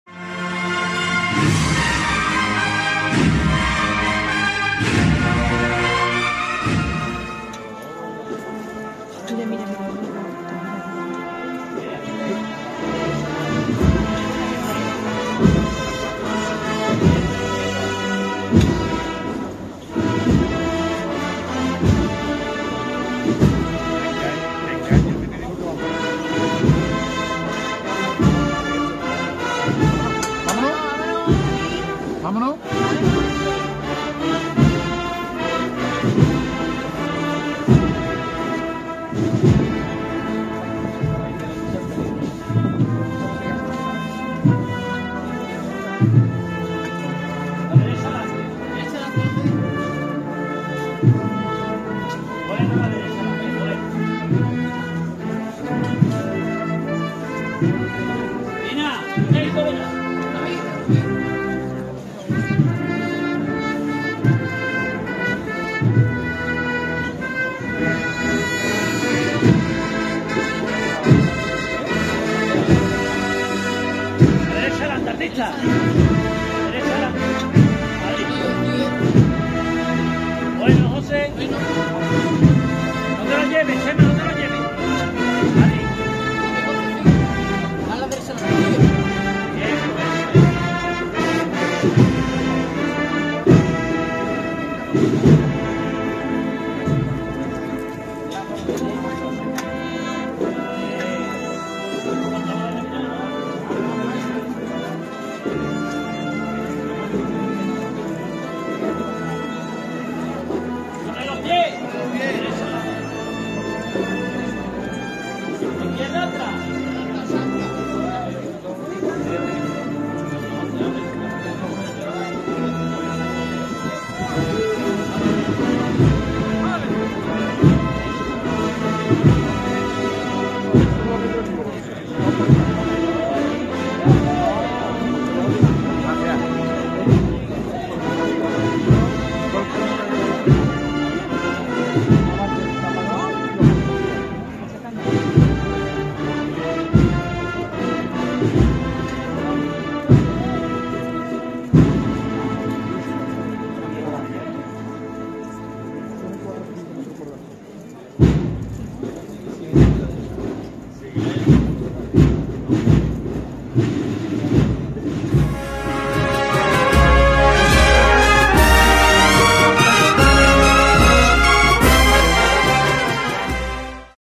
Exaltación (Las Viñas), calle Limones, Viernes Santo Jerez 2023
El paso de misterio del Santísimo Cristo de la Exaltación, titular cristífero de la Hermandad de las Viñas de Jerez, a su paso por la calle Limones; acompañado por la Agrupación Musical La Sentencia de Jerez, en la noche del Viernes Santo 7 de abril de 2023.